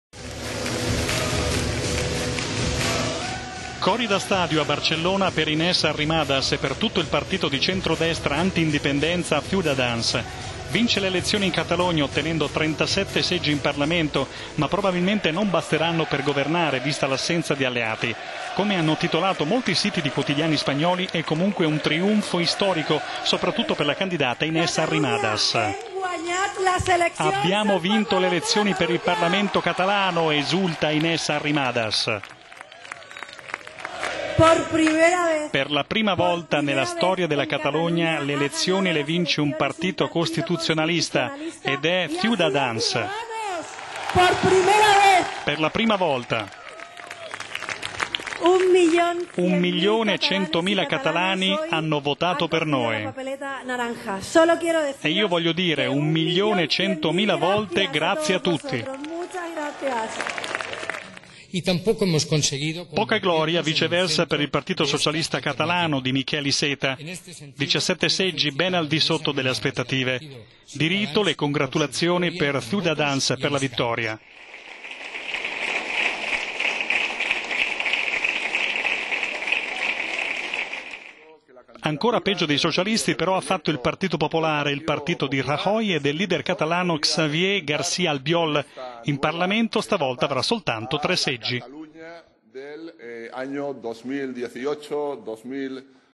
Elezioni in Catalogna: la "vittoria di Pirro" di Inés Arrimadas
La 36enne avvocatessa, candidata per Ciutadans, vince le elezioni, ma rischia di non riuscire ugualmente a formare il nuovo governo. Colpa della mancanza di alleati. Delusione elettorale per socialisti e popolari. …